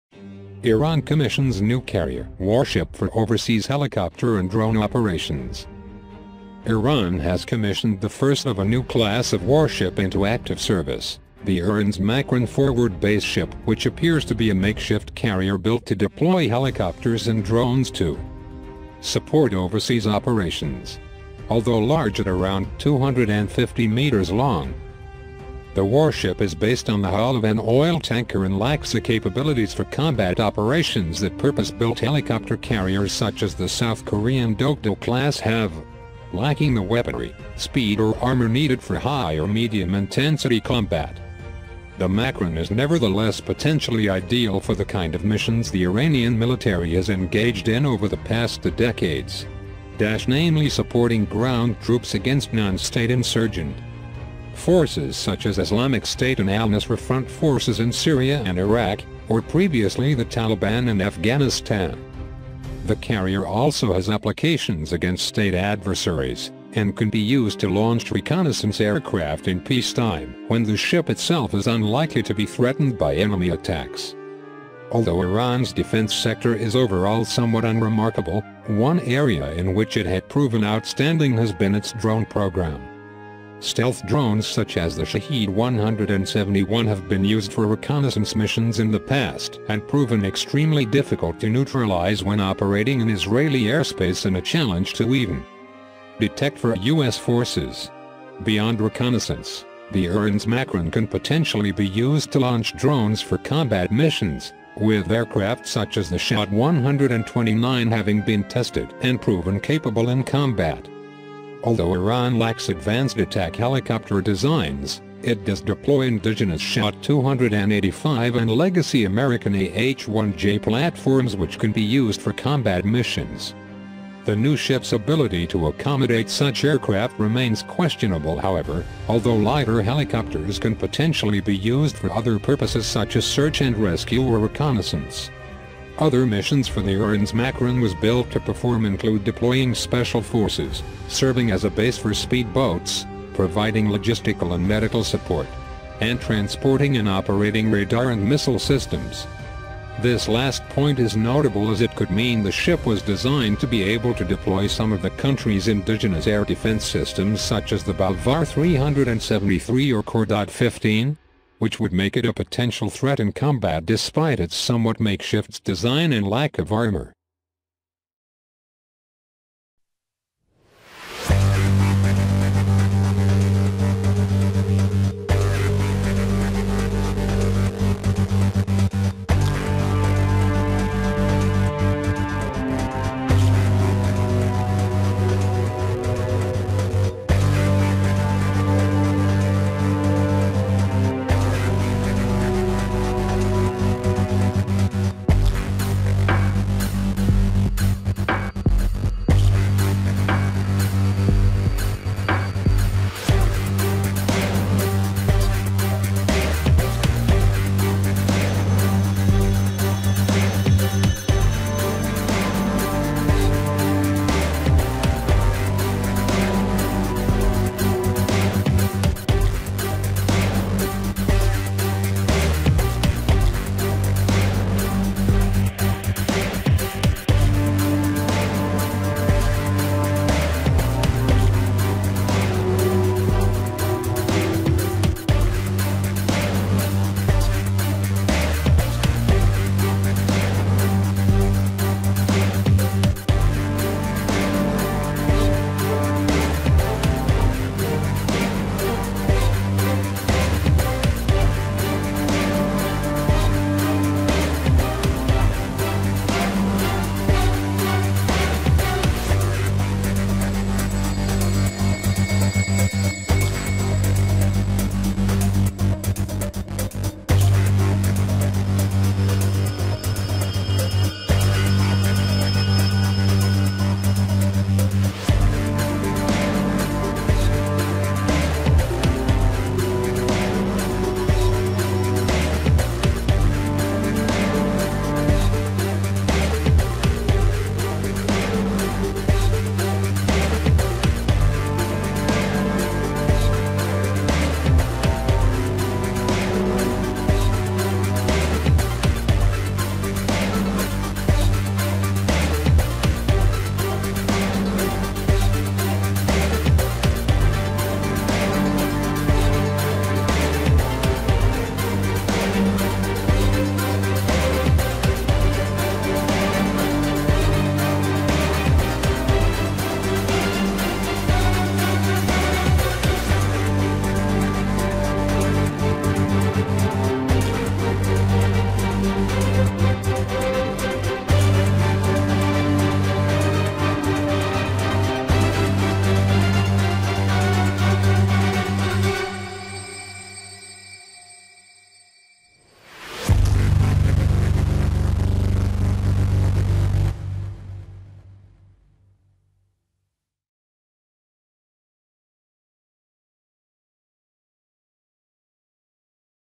Shocked Iran's New Nuclear-Powered Supercarrier, dubbed Project 23000E Shtorm (Storm)
Iran to build aircraft carrier for the strengthen its navy _ Iranian Navy 2021